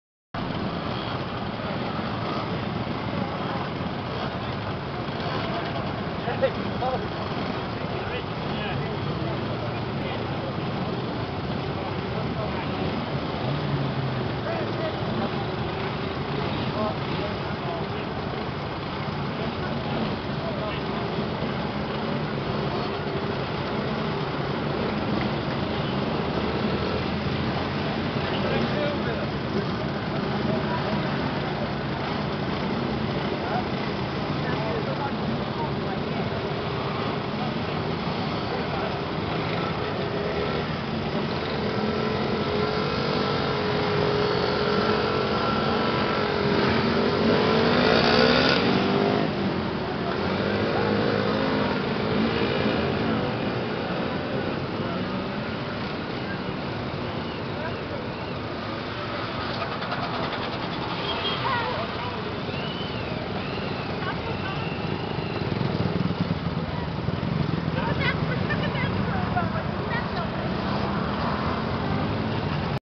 archerstown sept 19 2010 honda 50 run leaving petrol station
tanks full and we head off to do the rest of our run,this was a short run today(46 miles)and last 10 minutes rained so hard i was happy we were finished!!!